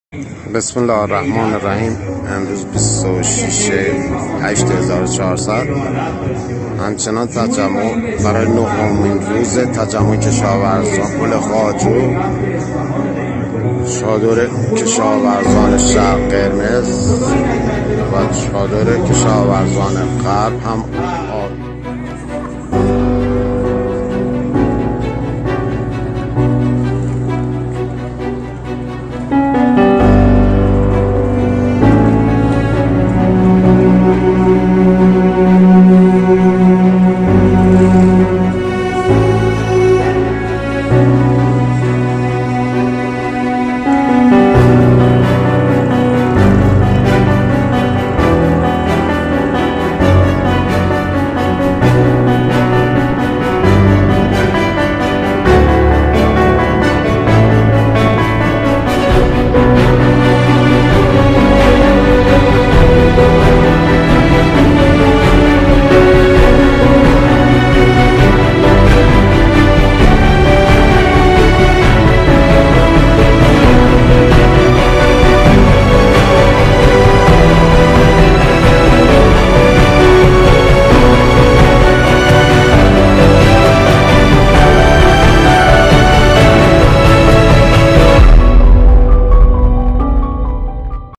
اخبار تصویری اعتراضات و اعتصابات ایران۲۷ آبان
▪️کشاورزان اصفهانی - ۱۰ روز اعتراض متوالی
▪️تجمع اعتراضی کارگران معدن مس دره‌زار روچون کرمان
▪️کارگران کارخانه لاستیک بارز سنندج
▪️کارگران کارخانه‌ سبلان پارچهٔ‌ اردبیل
▪️کارگران شرکت خدمات و ماشین‌آلات  صنعتى تراکتورسازی تبریز